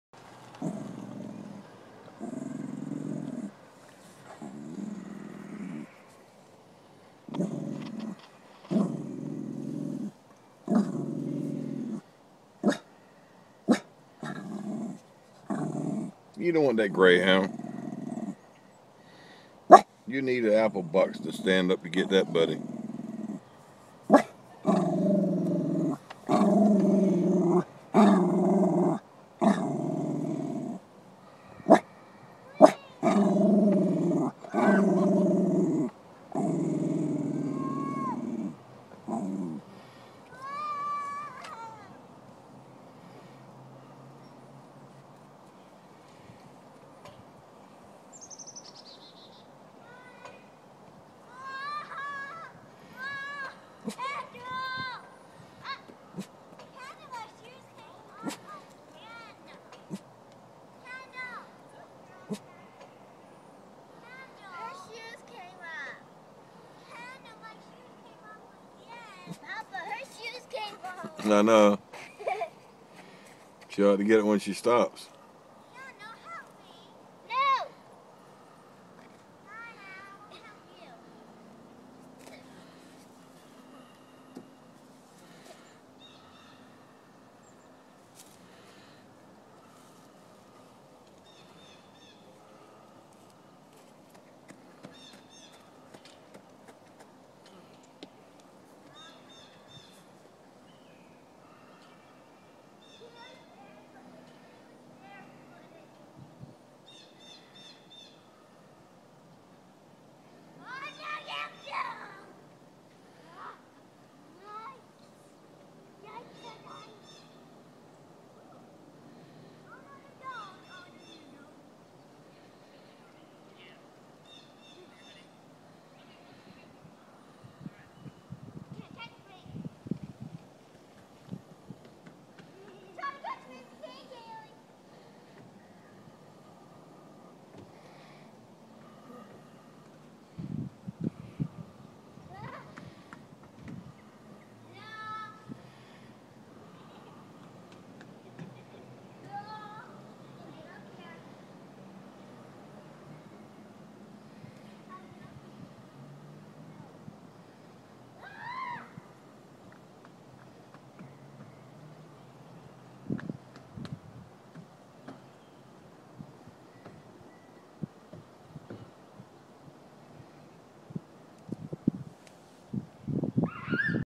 Day at the park with Papa
Kids at play BEANIE GROWLING AT A FEMALE GREYHOUND.HE MAY BE TRYING TO PICK HER UP.